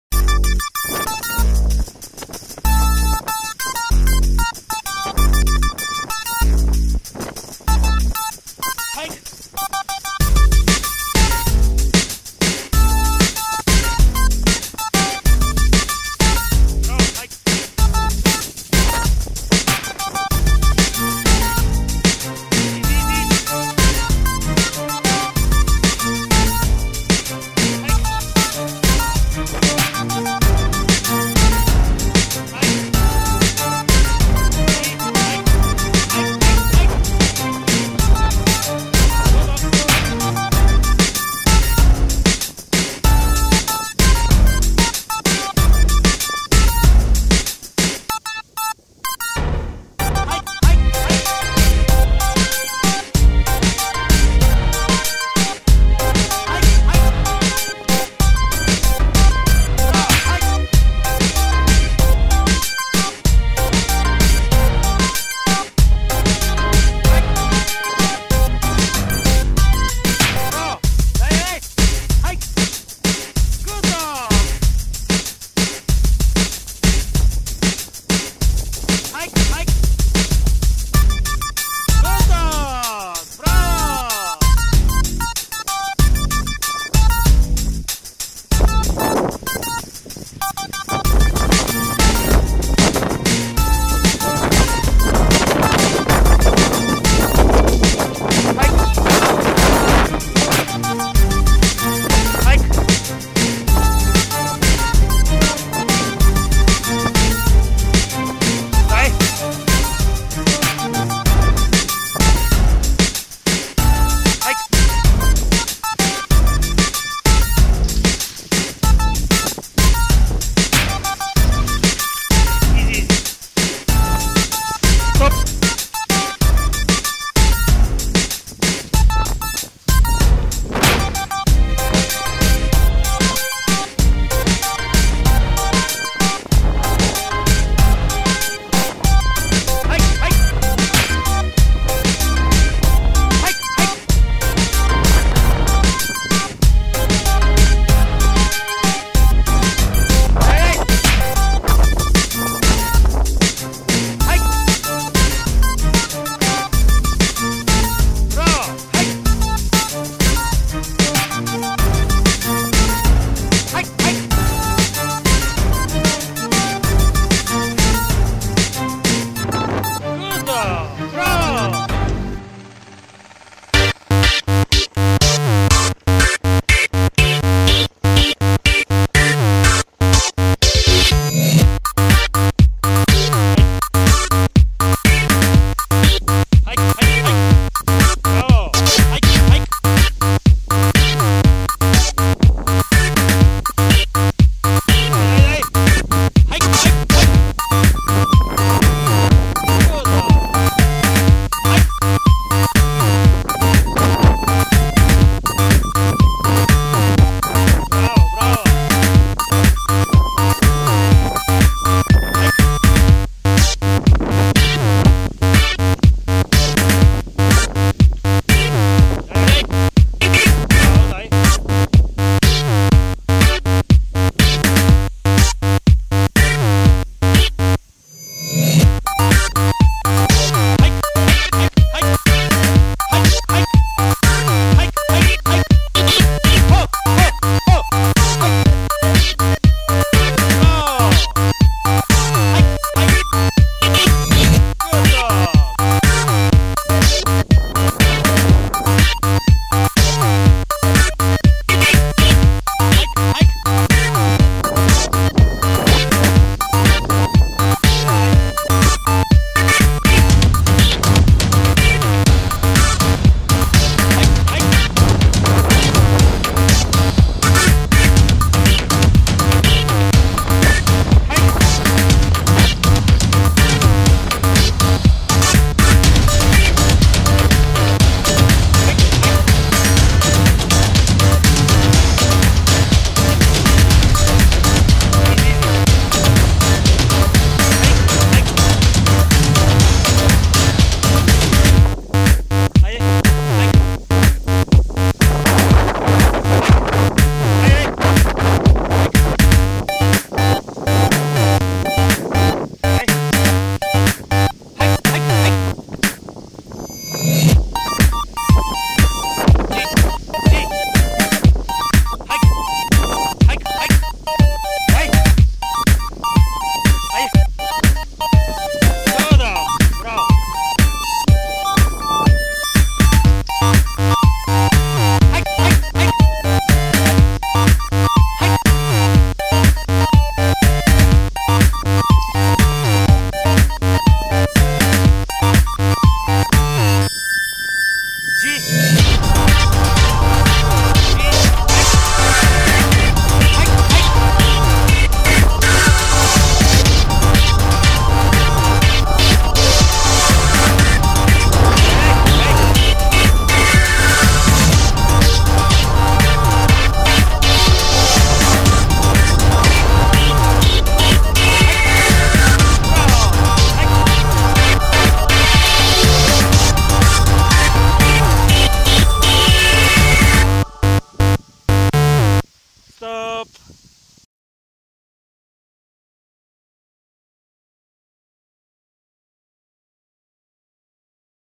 Skijoring with German Shepherd and a Labrador
Skijoring with German Shepherd and a Labrador acros Vitosha Mountain on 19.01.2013